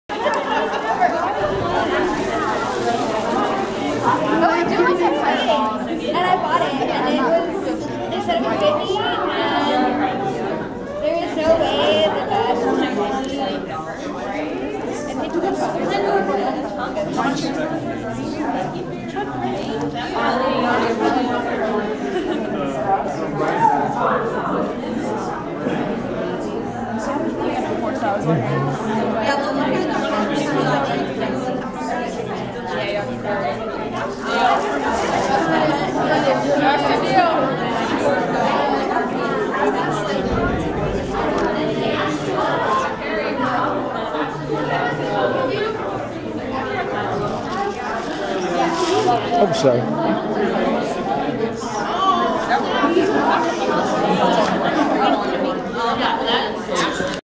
Cosplay and Costume Panel and exchange at the Phoenix Comicon 2014 Two parts